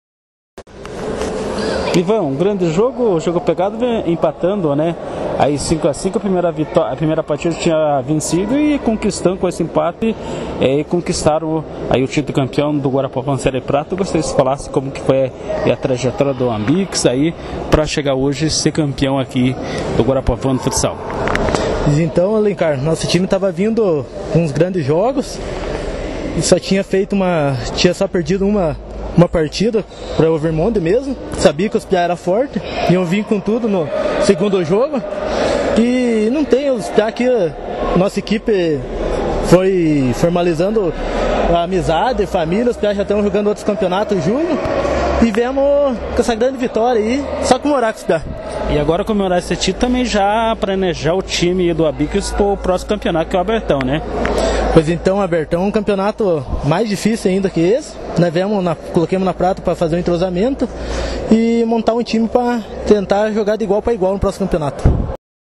Ambix conquista título da Prata no Guarapuavano de Futsal
Na noite desta terça-feira (27/08/19), a bola rolou no Ginásio Municipal Santa Terezinha pela grande final da categoria Adulto Prata do Campeonato Guarapuavano de Futsal 2019, a equipe do  Ambix empatou com o Virmond pelo placar de 05 a 05. Com a vitória na primeira partida a equipe do Ambix sagraram-se campeão da categoria Adulto Prata do Campeonato Guarapuavano de Futsal, temporada 2019. Acesse Blog//alencarsouza.blogspot.com.br